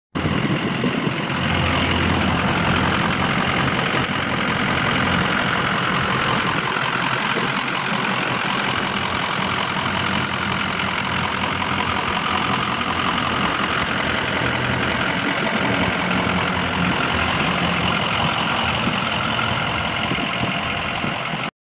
2006 Ford E450 cab and chassis for sale at auction | bidding closes May 22, 2019
2006 Ford E450 cab and chassis
85,274 miles on odometer
6.0L V8 OHV 32V diesel engine
Automatic transmission
Single axle
Suspension seat
Block heater
163" from rear of cab to end of frame
LT225/75R16 tires
AC does not blow cold, may need recharged
Decals have been removed
Kansas title